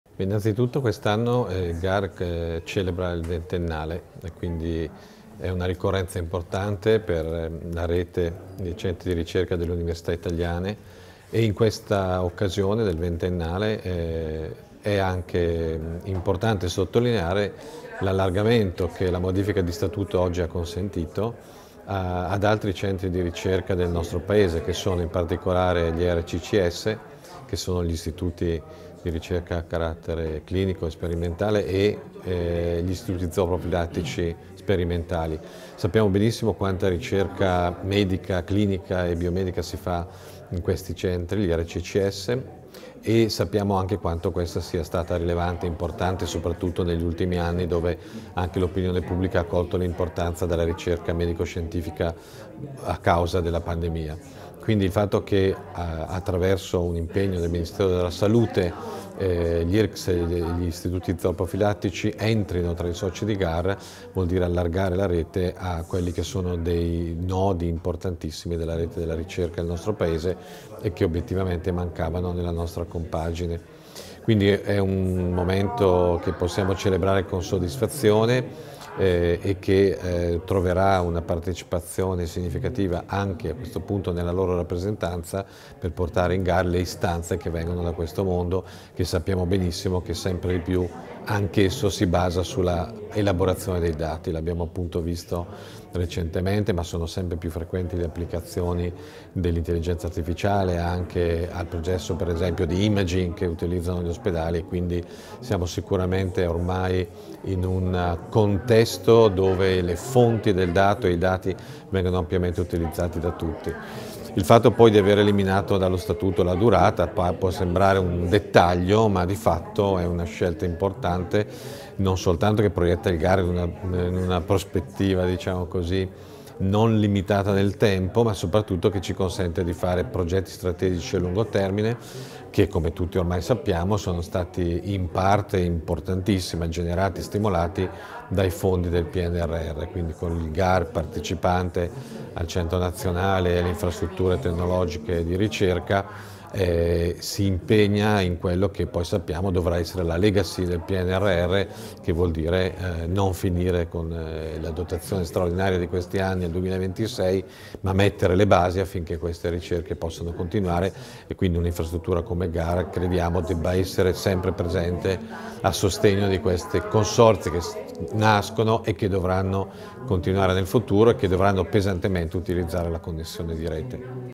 Nuovo statuto GARR - Maurizio Tira, Presidente GARR
Roma, 1 dicembre 2022 - È stato firmato stamattina il nuovo Statuto del Consortium GARR, l’associazione che gestisce la rete italiana dell’istruzione e della ricerca che proprio quest’anno celebra i suoi vent’anni di attività.

Intervista a Maurizio Tira, Presidente Consortiumm GARR